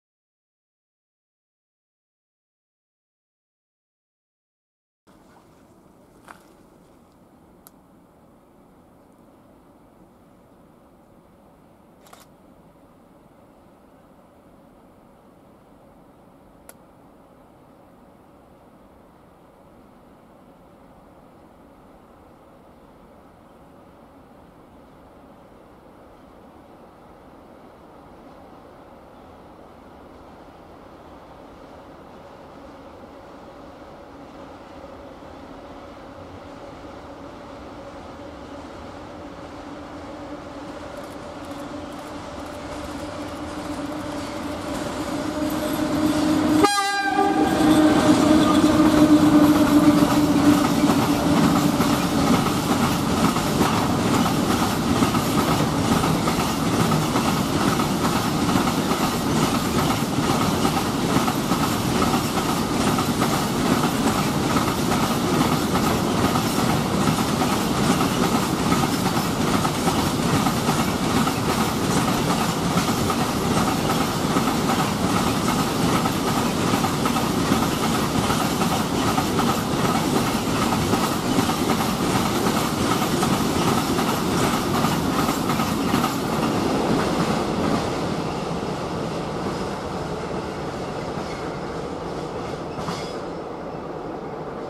ET22-988 z kontenerami na esce | Łętownia
OPIS
Siema!

Dziś film z  bykiem cargo wijącym się po łukach :p
SKŁADY
0:00 zdjęcie
0:05 ET22-988 z kontenerami w kierunku Przeworska
nagrane za pomocą:
Panasonic HC-V180